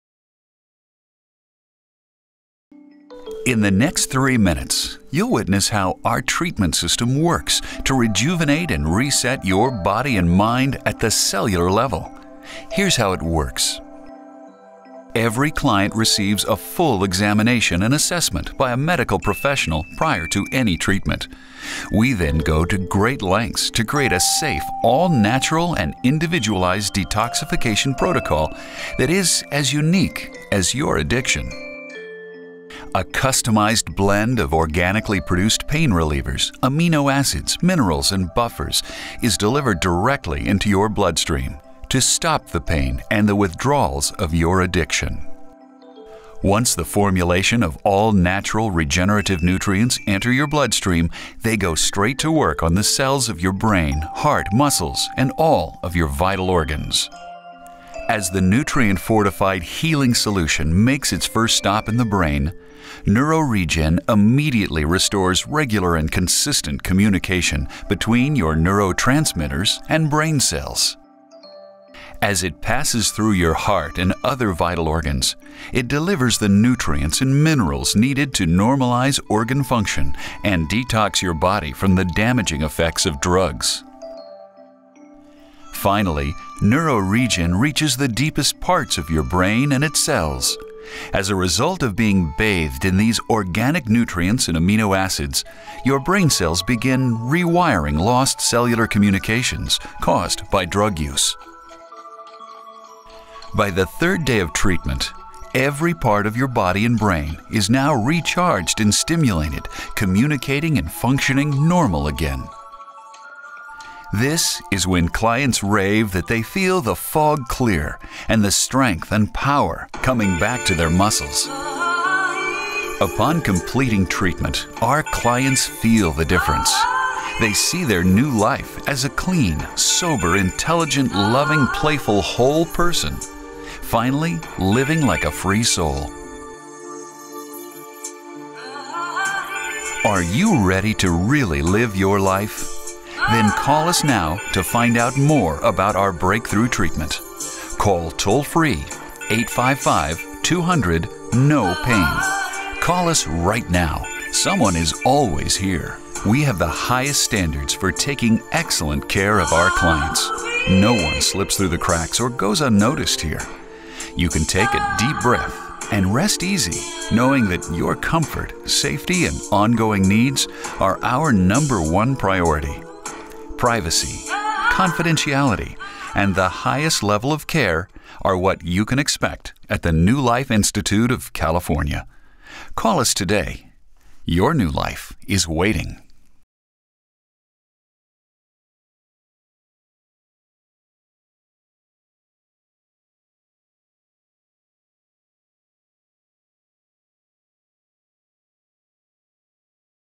NeuroRegen™ - How It Works
In 3 minutes, you'll witness how the NeuroRegen Infusion System works to rejuvenate and re-set your body and mind at the cellular level.   We go to great lengths to create a safe, all natural and individualized detoxification protocol that is as unique as your addiction. A customized blend of organically produced pain relievers, amino acids, minerals and buffers is delivered directly into your blood stream to stop the pain.. and the withdrawals of your addiction.  Once this formulation of all-natural regenerative nutrients enter your blood stream they go straight to work on the cells of your brain, heart, muscles and all of your vital organs.